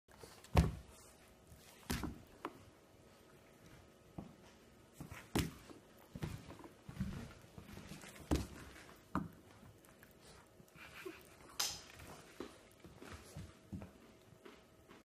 Golden Retriever Puppy Struggles with Carrot
This golden retriever puppy tries to figure out the nimble movements of a carrot...